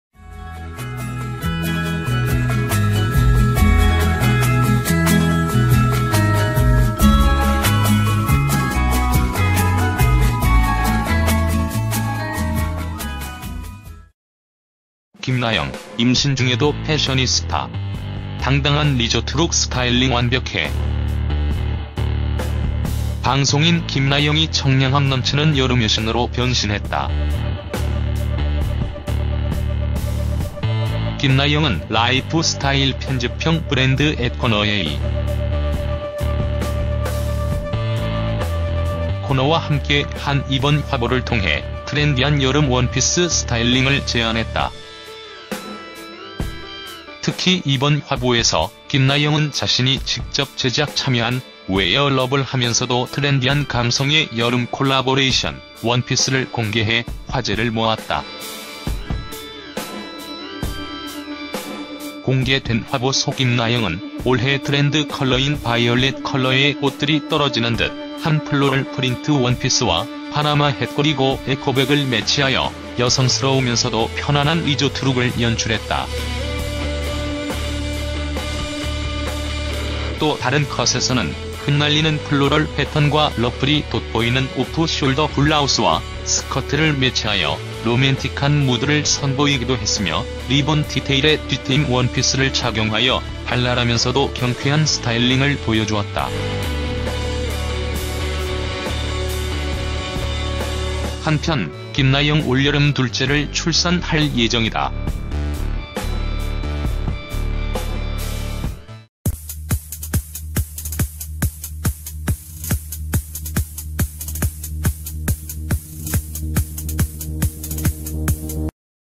김나영, 임신중에도 패셔니스타! 당당한 리조트룩 스타일링 '완벽해'
방송인 김나영이 청량함 넘치는 여름 여신으로 변신했다.